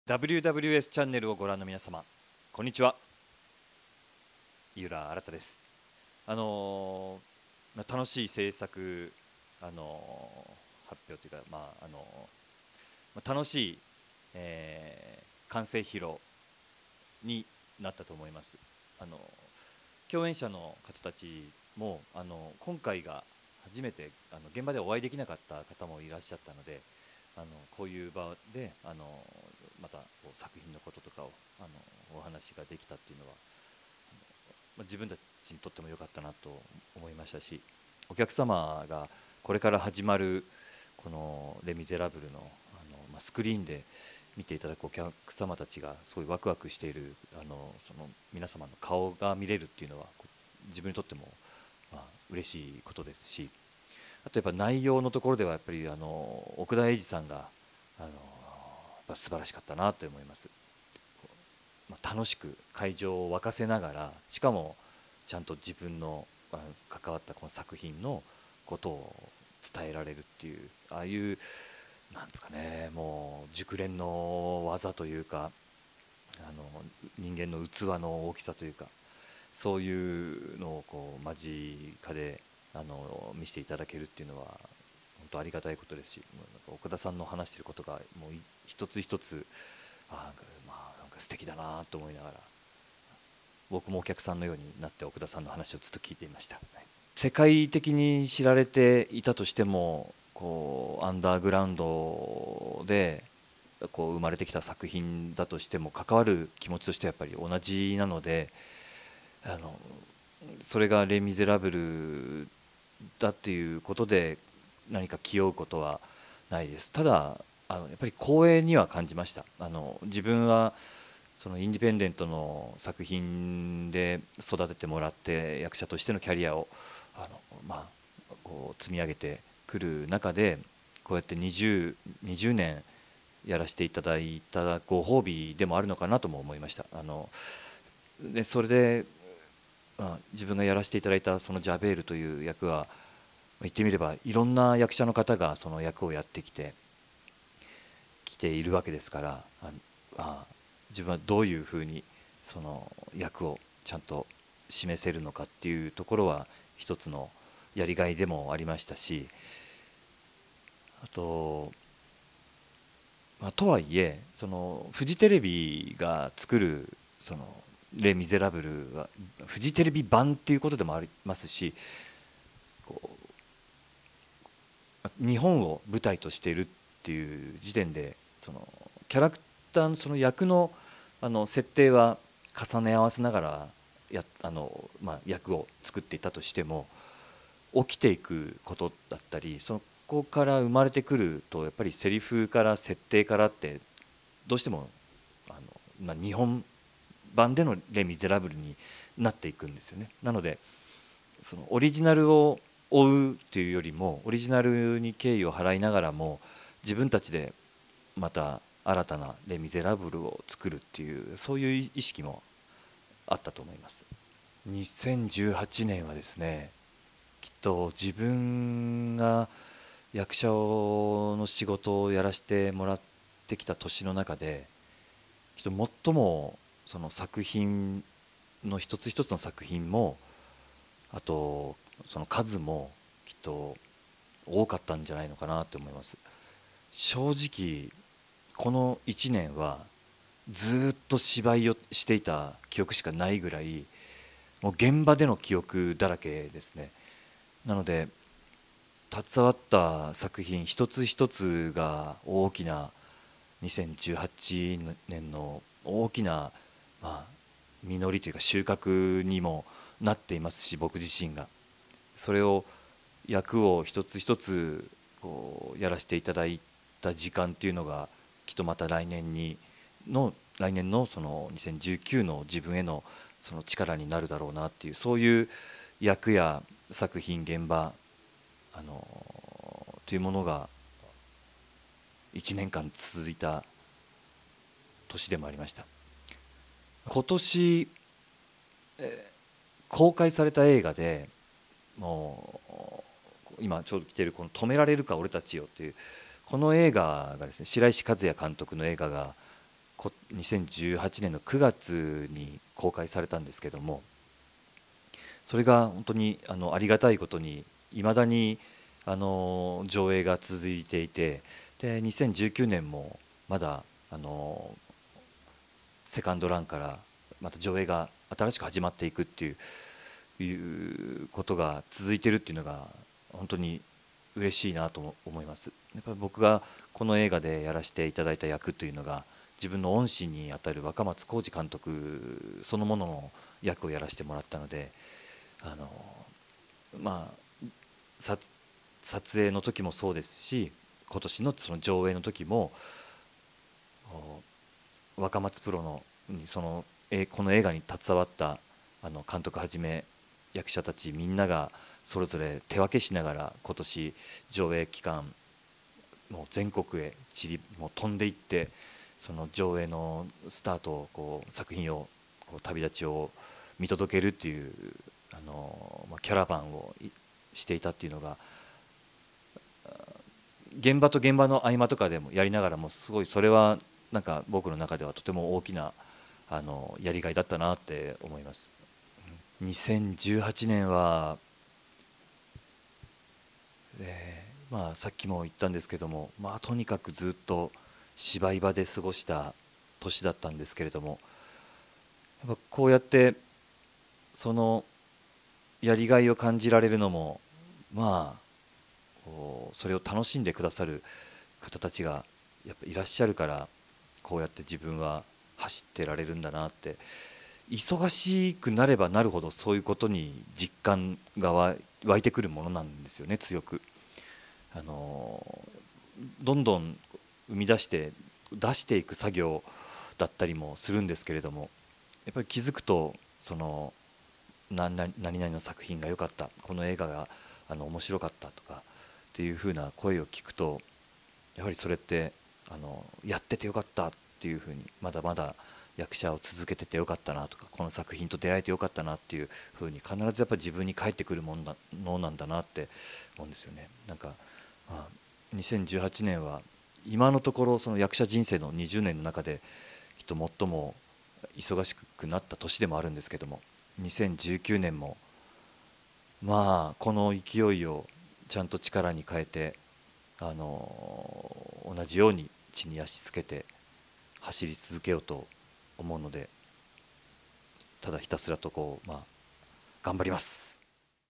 井浦新がフジテレビドラマ『レ・ミゼラブル 終わりなき旅路』主演へ コメント！2018年『アンナチュラル」を振り返り俳優人生に感謝！
12月25 日、俳優の井浦新が2019年1月6日に放送されるフジテレビ開局60周年特別ドラマ『レ・ミゼラブル  終わりなき旅路』の制作発表会に出演した後、WWSチャンネルのインタビューに応じた。

井浦は今回ディーン・フジオカとのダブル主演をつとめるが、ドラマのみどころから2018 年大ブレイクした『アンナチュラル』を振り返って語ってくれた。